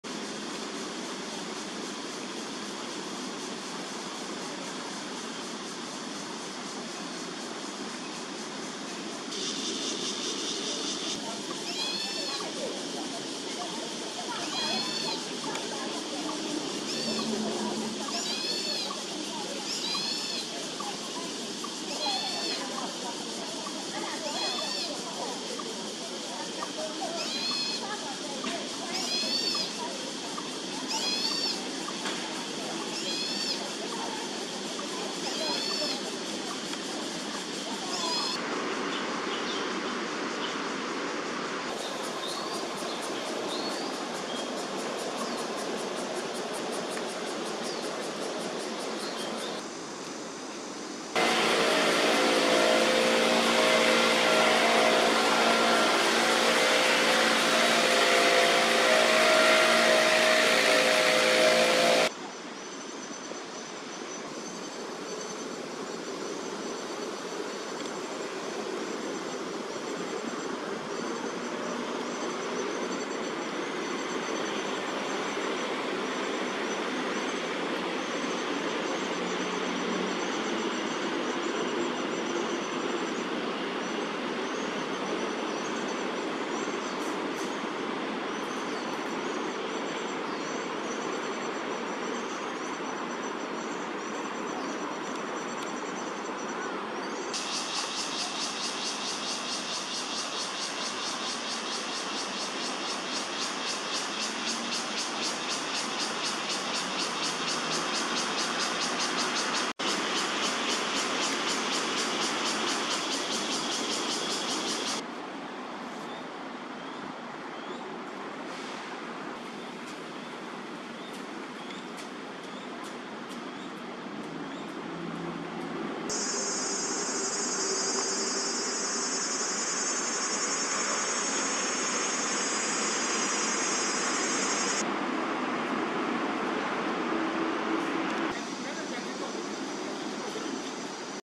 20230704 〈南大安〉廣志爸曬翅～〈北大安水花家〉藍H1小朋友和小花媽站同樹【大安森林公園鳳頭蒼鷹Crested Goshawk】
南大安廣志鷹爸站在高處張望. 後來換到附近樹上曬翅. 今年的巢區，有巢黑冠正在孵蛋中。北大安水花家，藍H1小朋友待在蔭涼處東張西望著. 後來，小花媽飛來了！小朋友跑來和媽媽站在同棵樹上。母女倆就這樣同樹待了好久. 中間小花媽幾度只是動了一下，一邊的藍H1就開始叫起來.

0:00 廣志鷹爸站高處張望（南大安）
0:11 廣志鷹爸曬翅（南大安）
0:38 黑冠麻鷺臥巢中（南大安）
0:42 樹上松鼠活動中（北大安水花家）
0:49 鴿子趴地上（北大安水花家）
0:51 藍H1小朋友站蔭涼處張望著（北大安水花家）
1:02 小花媽現身，母女倆站同樹（北大安水花家）